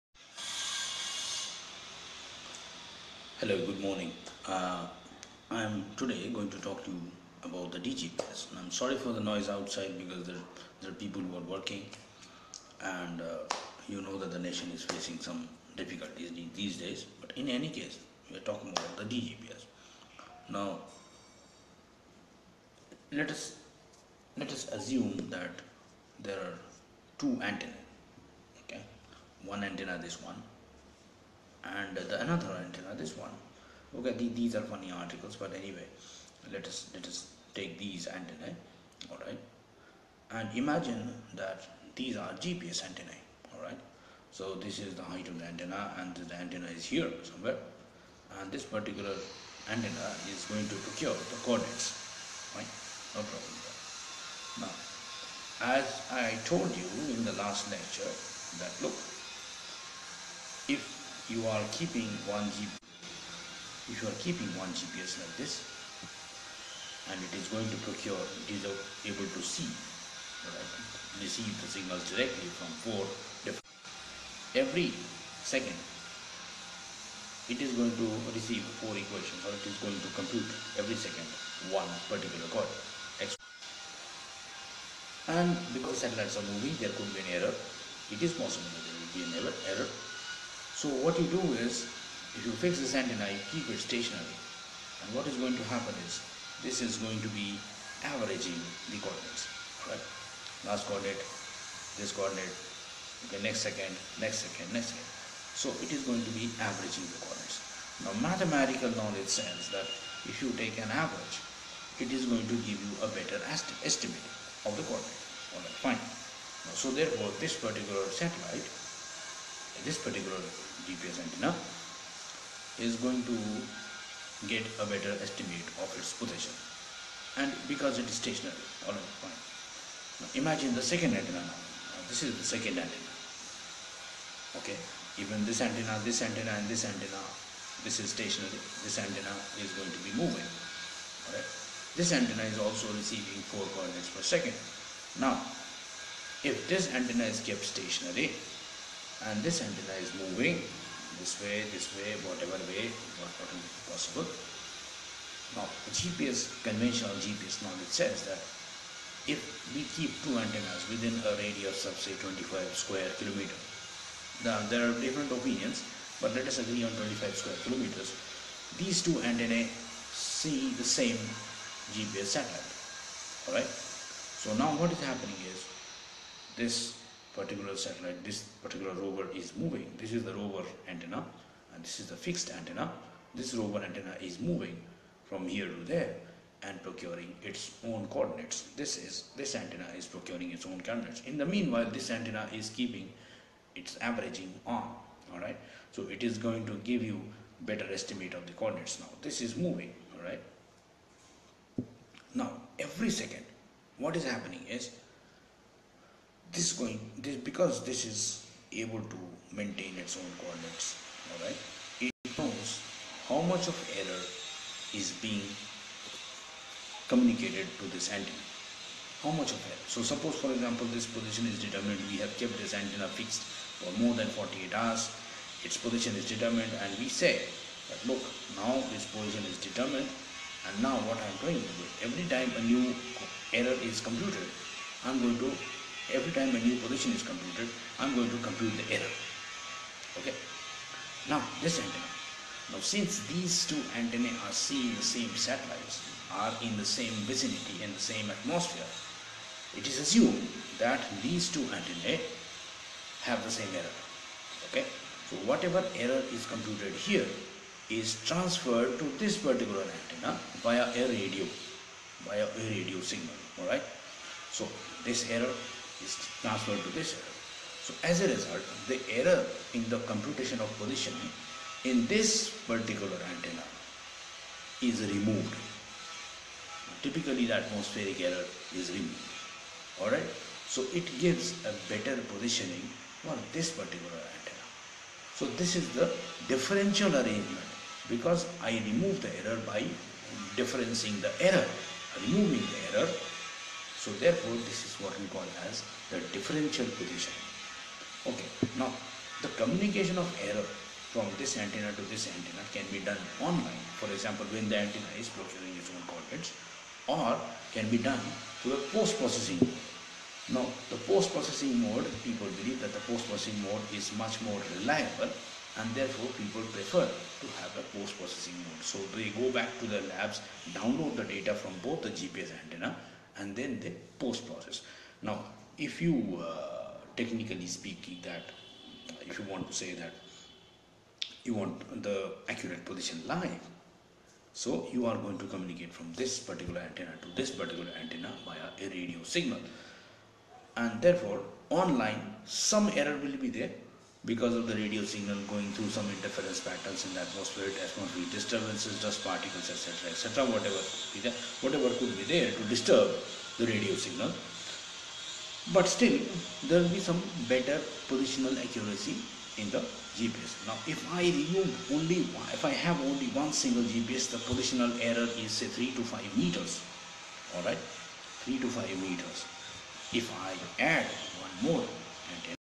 DGPS
This video tells you, in simple language, as to how the differential positioning works.